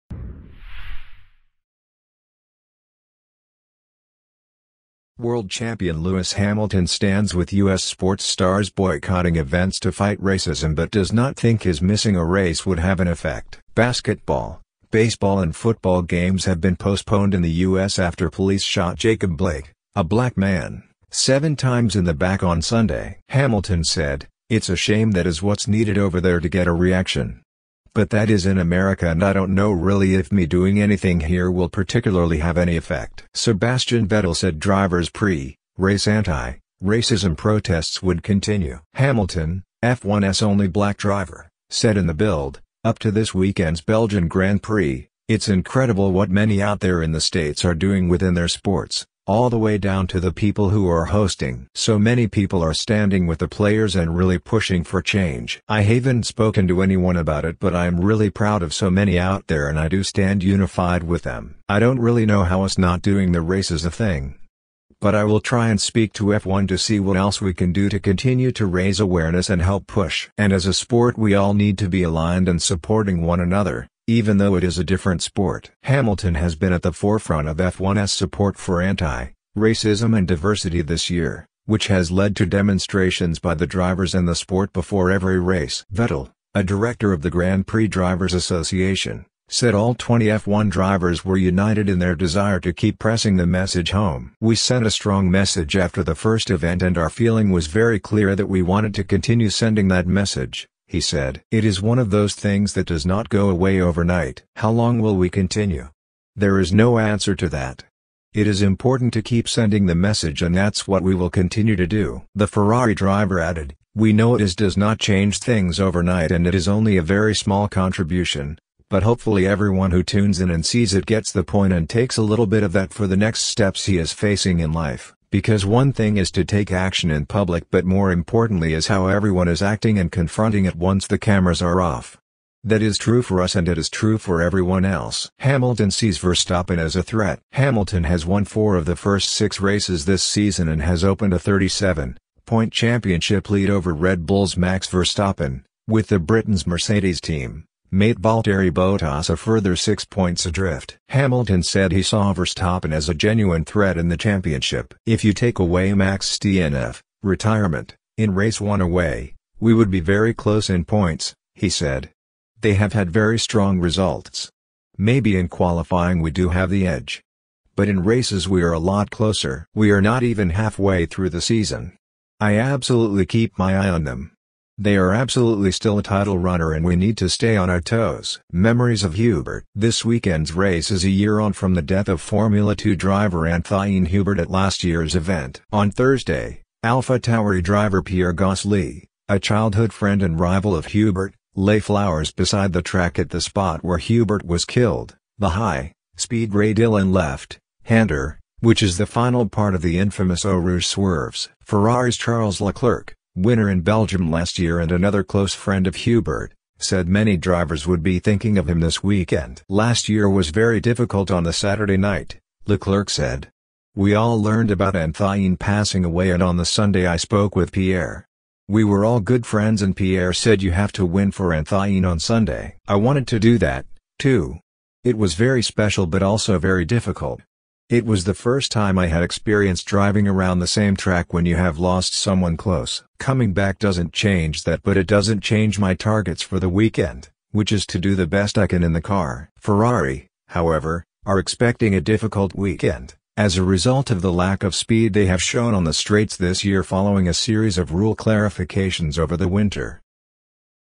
Belgian GP: Lewis Hamilton rules out race boycott - News Today
-- News Today --